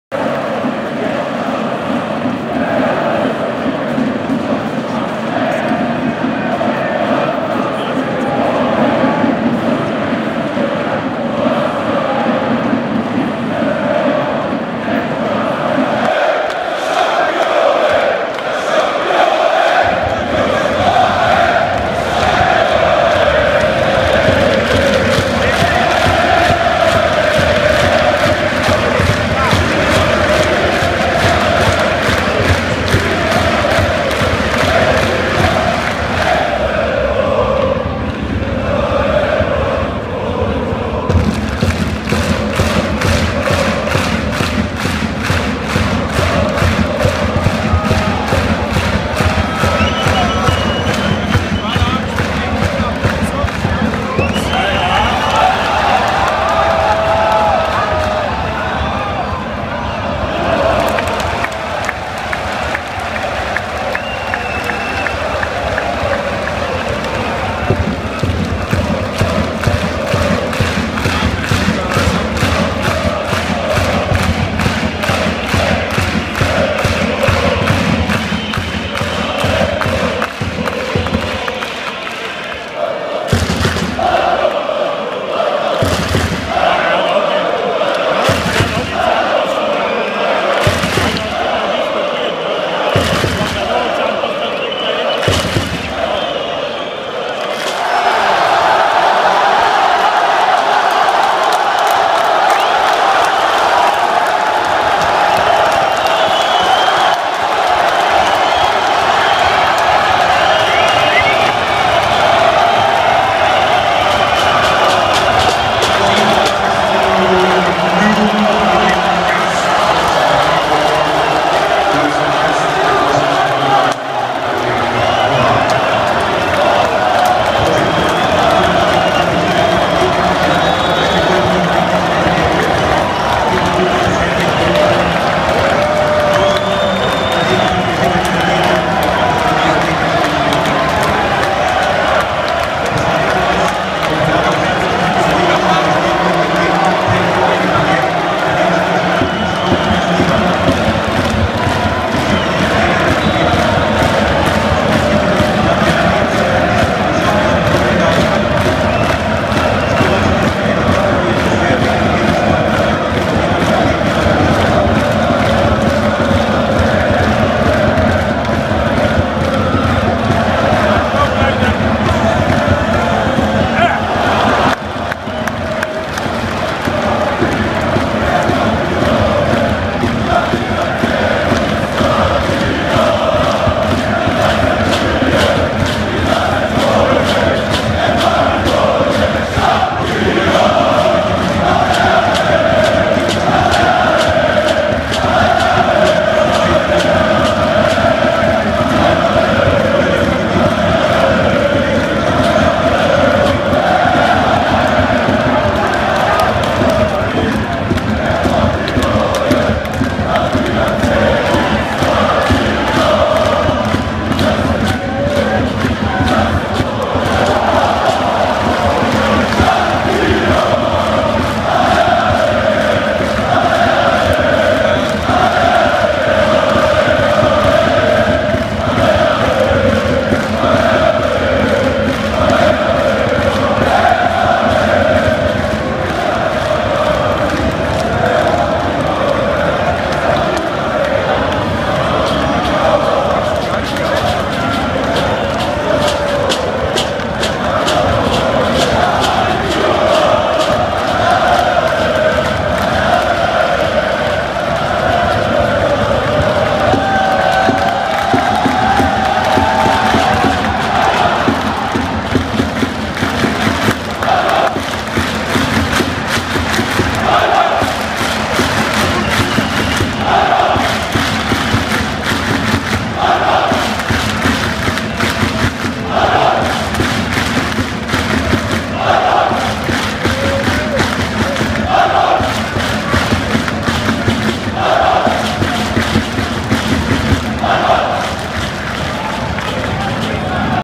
Schalke - NK Maribor -- Slowenischer Support
Das 1:1 zwischen Schalke 04 und NK Maribor im zweiten Gruppenspiel der Champions-League-Saison 2014/15 sahen 49.997 Fans in der Veltins Arena Gelsenkirchen. Darunter waren etwa 1300 Gästefans, die einen ordentlichen Support ablieferten, aber das Tor zum 1:0 mit Pyro feierten. Dabei fackelten sie fast ihre eigene Zaunfahne ab. So sah das aus. (Mehr zum Spiel und zu Schalke im Blog